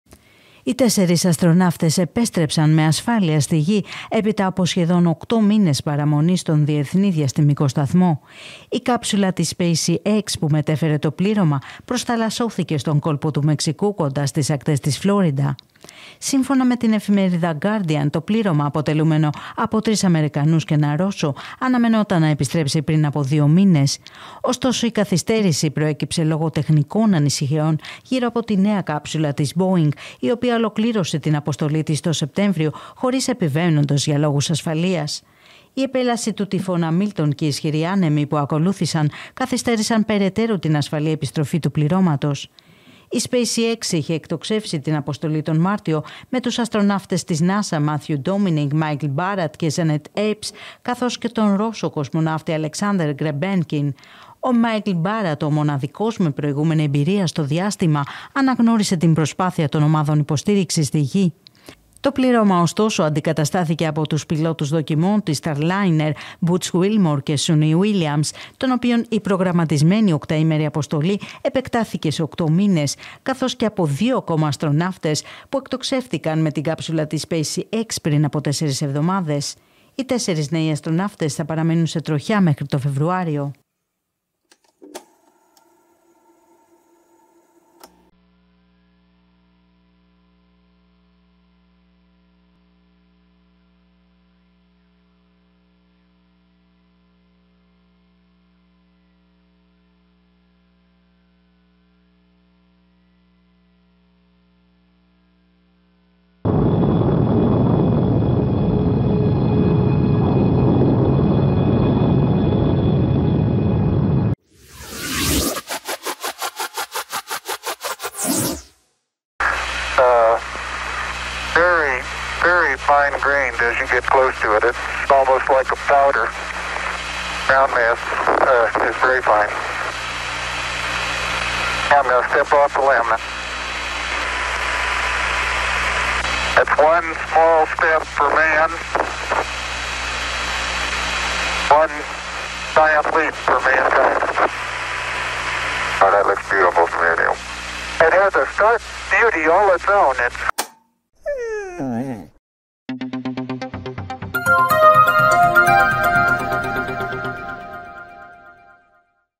«SpaceX»: Επέστρεψαν στη Γη οι 4 αστροναύτες | Pronews TV
Οι τέσσερις αστροναύτες επέστρεψαν με ασφάλεια στη Γη, έπειτα από σχεδόν οκτώ μήνες παραμονής στον Διεθνή Διαστημικό Σταθμό.

Η παραμονή τους στον Σταθμό είχε παραταθεί, εξαιτίας τεχνικών προβλημάτων στην κάψουλα Starliner της Boeing και των ακραίων καιρικών φαινομένων από τον τυφώνα Μίλτον.

Η κάψουλα της «SpaceX» που μετέφερε το πλήρωμα προσθαλασσώθηκε στον Κόλπο του Μεξικού, κοντά στις ακτές της Φλόριντα, τα ξημερώματα της Παρασκευής.

Σύμφωνα με τον Guardian, το πλήρωμα, αποτελούμενο από τρεις Αμερικανούς και έναν Ρώσο, αναμενόταν να επιστρέψει πριν από δύο μήνες. Ωστόσο, η καθυστέρηση προέκυψε λόγω τεχνικών ανησυχιών γύρω από την νέα κάψουλα της Boeing, η οποία ολοκλήρωσε την αποστολή της τον Σεπτέμβριο χωρίς επιβαίνοντες για λόγους ασφαλείας. Η επέλαση του τυφώνα Μίλτον και οι ισχυροί άνεμοι που ακολούθησαν καθυστέρησαν περαιτέρω την ασφαλή επιστροφή του πληρώματος.